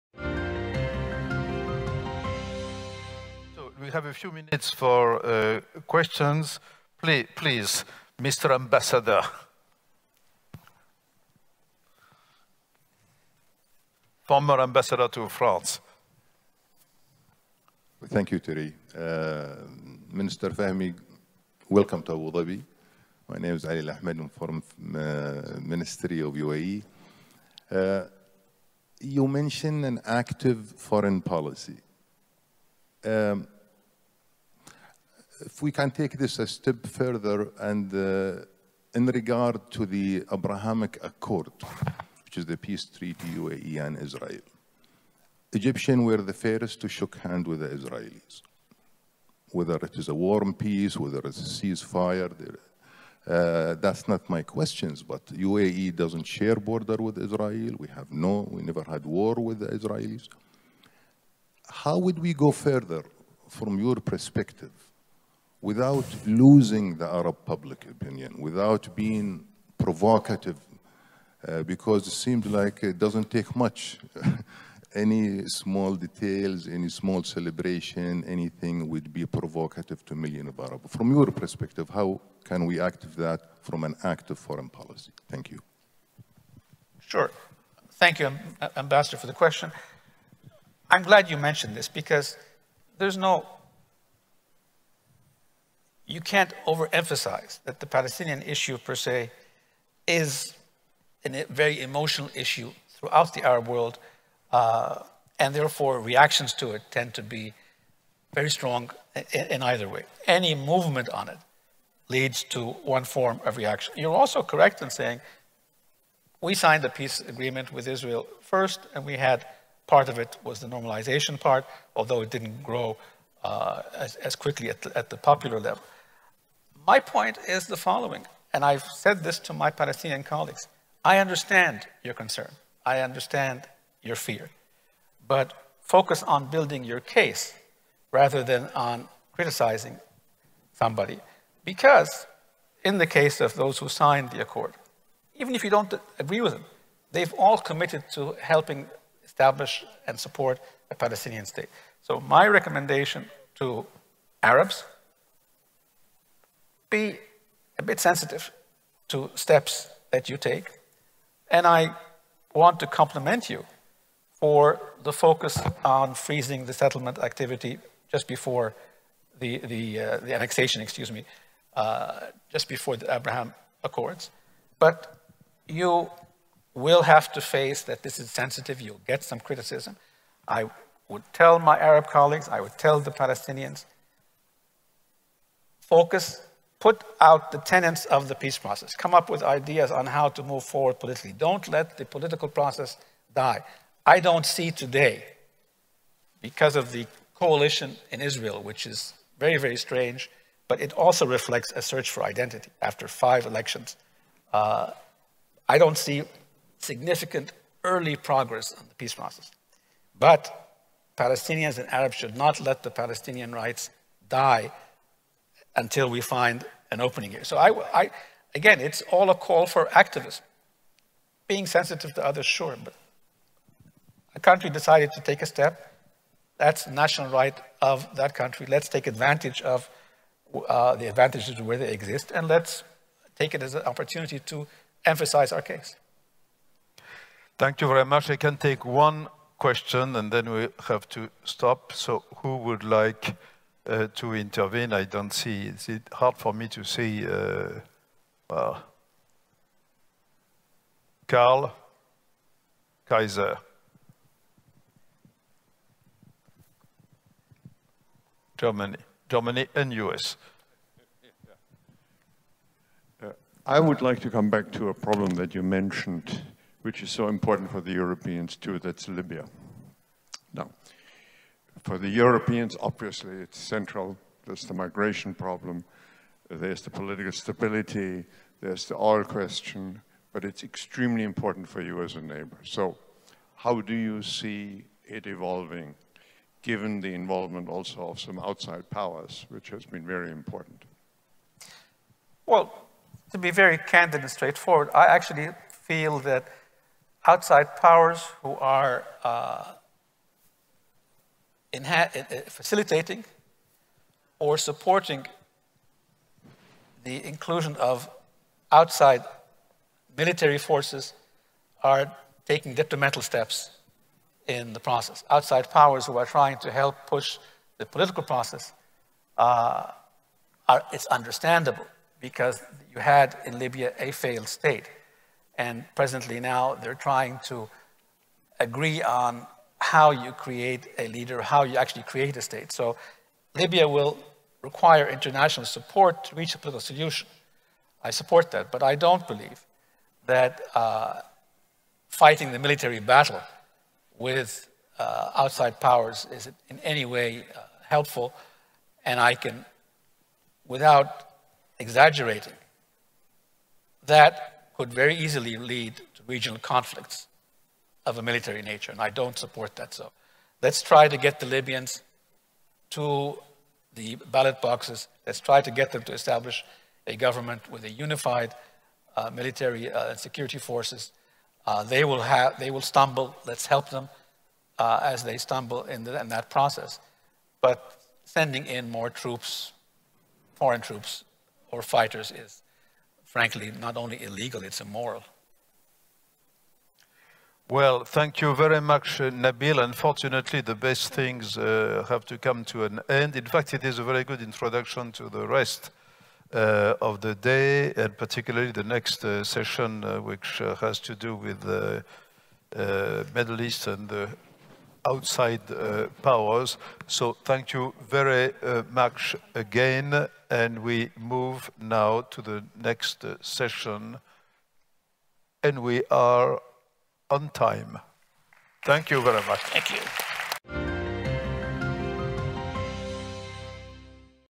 WPC 2021 - Debate - Plenary session 13: Conversation with Nabil Fahmy
Thierry de Montbrial is the Founder and Chairman of Ifri and the WPC
Nabil Fahmy is the  Founding Dean of the School of Global Affairs and Public Policy at the American University in Cairo, former Foreign Minister of Egypt